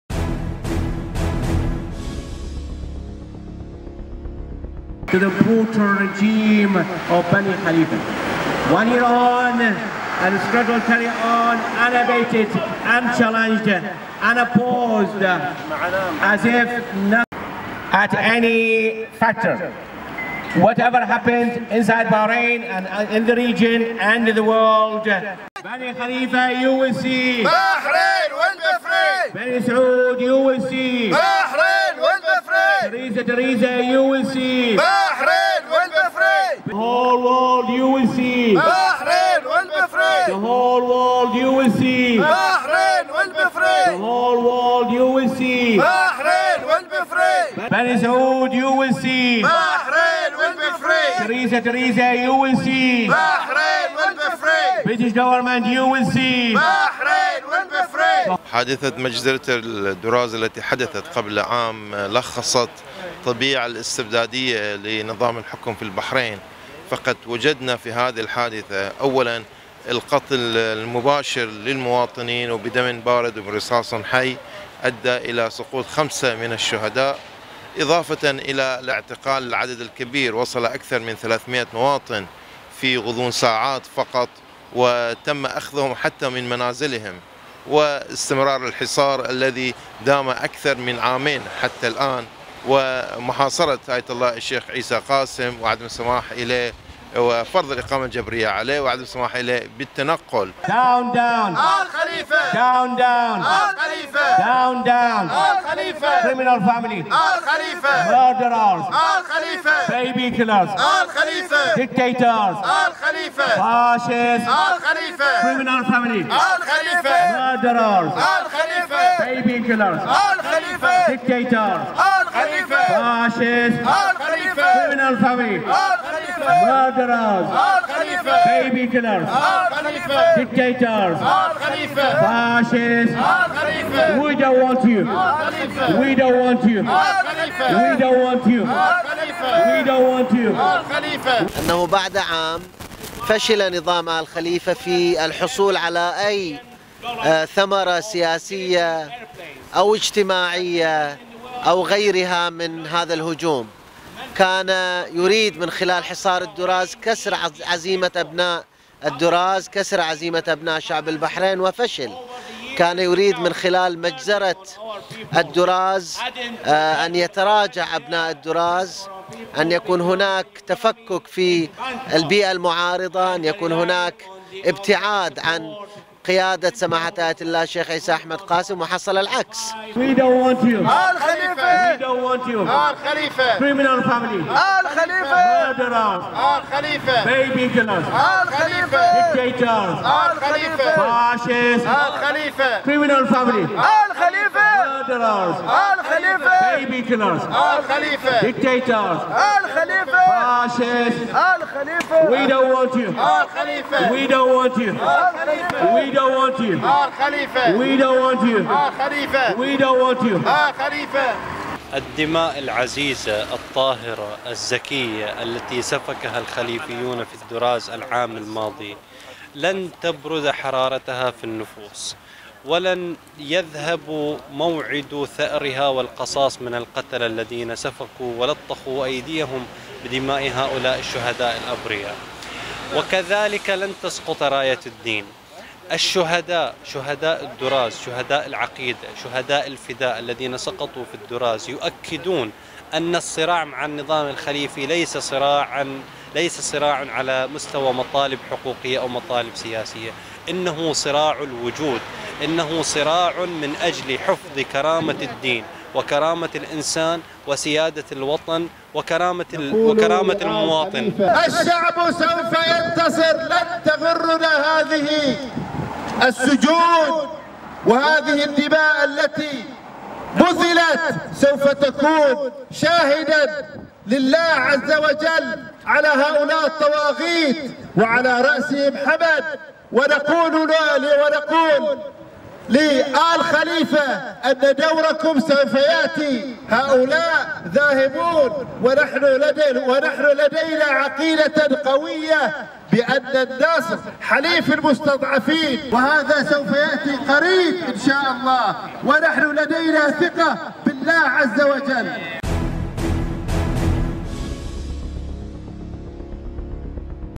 شاهد: اعتصام احتجاجي أمام السفارة الخليفية في لندن بذكرى الهجوم الدموي على معتصمي الدراز
لندن – البحرين اليوم

نظم تكتل المعارضة البحرانية في بريطانيا اليوم الجمعة ١٨ مايو ٢٠١٨م اعتصاما احتجاجيا أمام السفارة الخليفية في العاصمة لندن بمناسبة قرب الذكرى السنوية الأولى للهجوم الدموي على المعتصمين بجوار منزل آية الله الشيخ عيسى قاسم في بلدة الدراز.

ورفع المعتصون لافتة كبيرة عليها صورة الشيخ قاسم، وهتفوا بشعارات دانت استمرار الحصار على منزل الشيخ قاسم ومحاصرة بلدة الدراز، ودانوا الدعم البريطاني والأمريكي للنظام الخليفي والتغطية على جرائمه المتواصلة بحق المواطنين والنشطاء.

وأشار المعتصمون إلى أن الحصار المفروض على الشيخ قاسم والعقاب الجماعي على أهالي الدراز يأتي في سياق مشروع “الاضطهاد الطائفي” الممنهج الذي يمارسه النظام بحق السكان الأصليين في البلاد، ودانوا الصمت الدولي عما يجري في البحرين، بما في ذلك عمليات القتل والتعذيب والاعتقالات الواسعة.